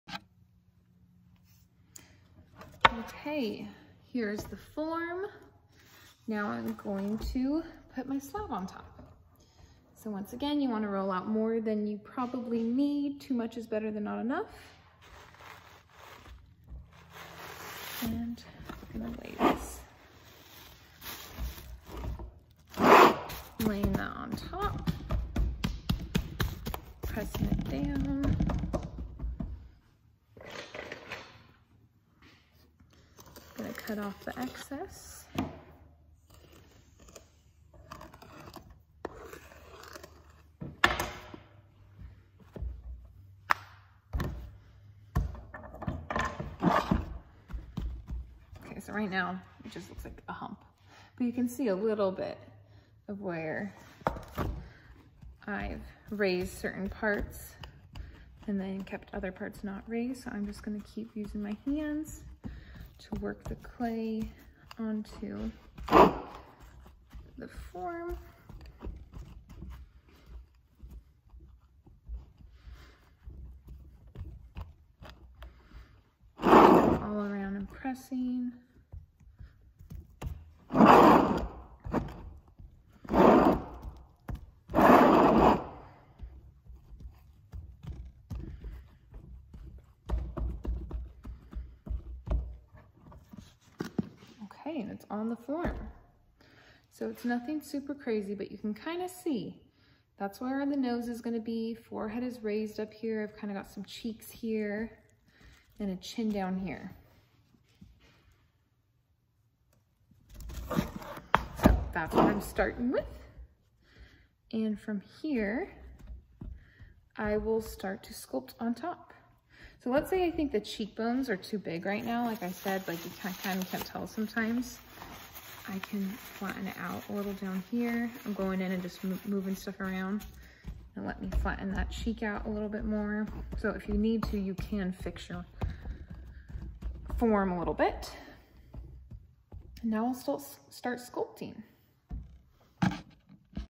Mask Project: Form #2
How to put your slab on top of your form.